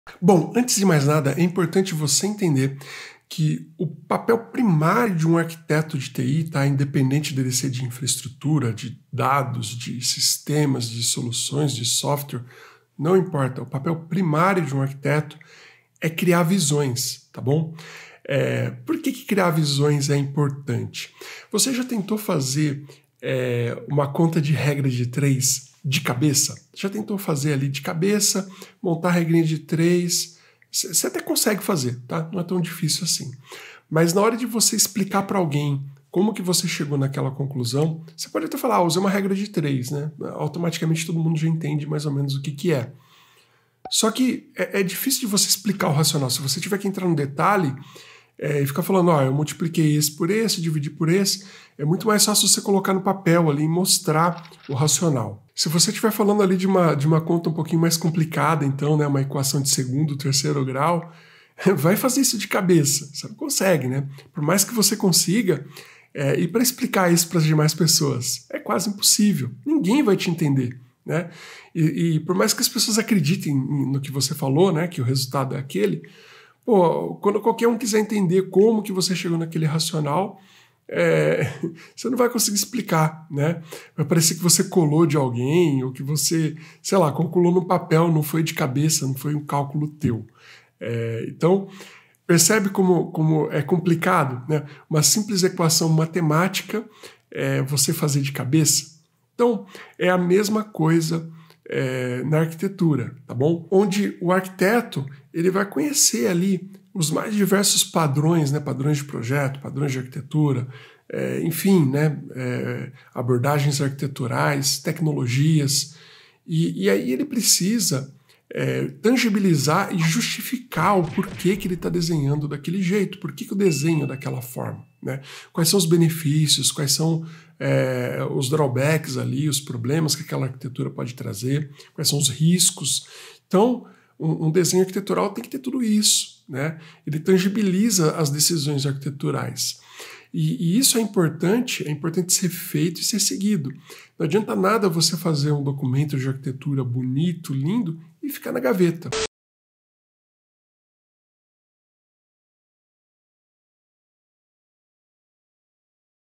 Descubra qual a principal responsabilidade de Arquitetos de TI
Os Arquitetos de TI são responsáveis ​​por compreender os sistemas tecnológicos complexos logo abaixo do nível da aplicação e desenvolver soluções que melhorem a capacidade de dados, segurança e funcionalidade de um sistema. Eles também são responsáveis ​​por fornecer arquiteturas de sistemas de software e hardware que oferecem a escalabilidade necessária para TI. Além disso, os Arquitetos de TI também são responsáveis ​​pela documentação de todos os processos, projetos e design de hardware, a fim de assegurar o pleno cumprimento de políticas de TI.

Mas existe uma responsabilidade crucial para qualquer um dos tipos de arquitetos, sejam de Arquitetos de Software, Arquitetos de Sistema, Arquitetos de Soluções ou qualquer outro tipo de arquiteto tecnológico em TI. Todos eles têm como principal atividade a elaboração de visões, sejam diagramas, documentos ou desenhos na em uma lousa...o que conta é o arquiteto crie visões para tangibilizar as suas soluções a justificativa do uso de técnicas, abordagens, padrões ou tecnologias.

DICAS DE VÍDEOS PARA VOCÊ MARATONAR:

▶️ Entenda a relação entre RAs, VAs com o que há de comum entre os tipos de Arquitetos de TI